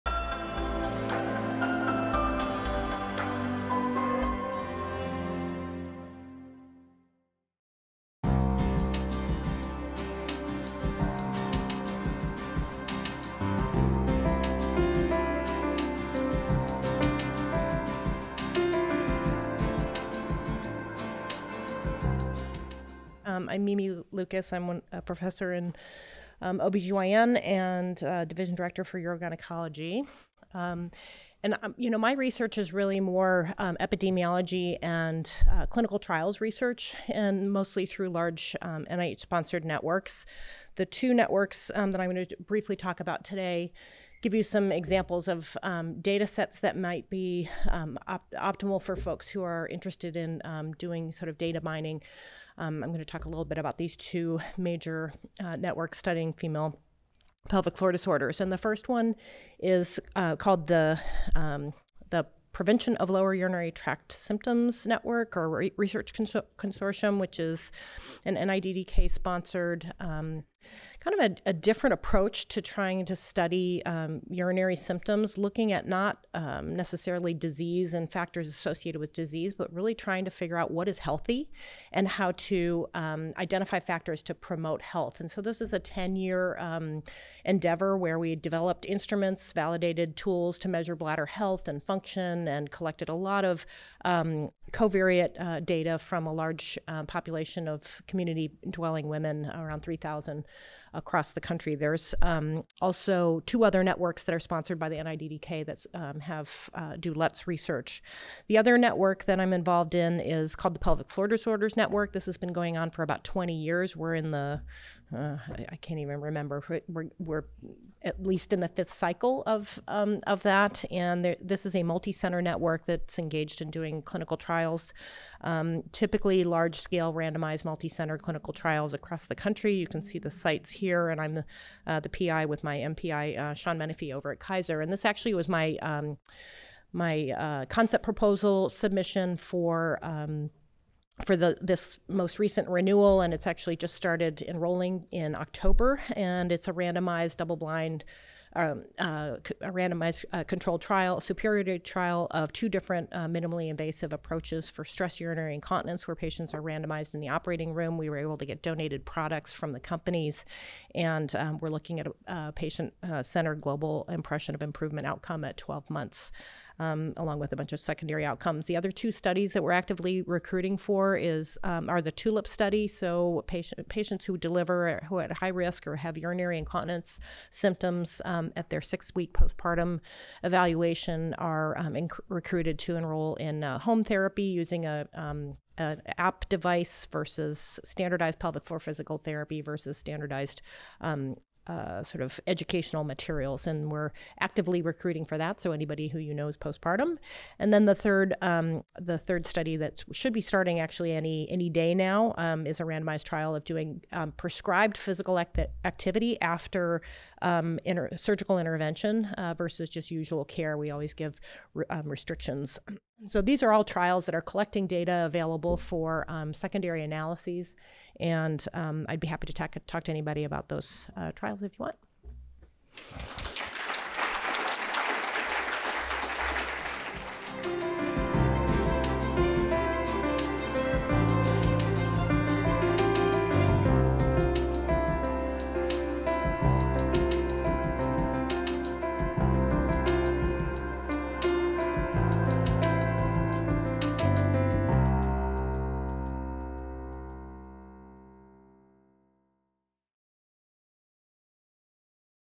Advancing Pelvic Floor Disorder Research: NIDDK & NICHD Networks
Emily Lukacz, M.D., M.A.S. presents research from two national networks focused on female pelvic floor disorders. One project studies bladder health in over 3,000 women to identify factors that support wellness rather than disease. Another conducts large-scale clinical trials, including a randomized study comparing two minimally invasive treatments for stress urinary incontinence. Additional trials include the TULIP study, which evaluates postpartum urinary symptom treatments using an app-based device, physical therapy, or educational materials, and a new study testing prescribed physical activity after pelvic surgery. These studies emphasize patient-centered outcomes and generate data available for further research and analysis. [7/2025] [Show ID: 40769]

More videos from: UC San Diego Women's Health Symposium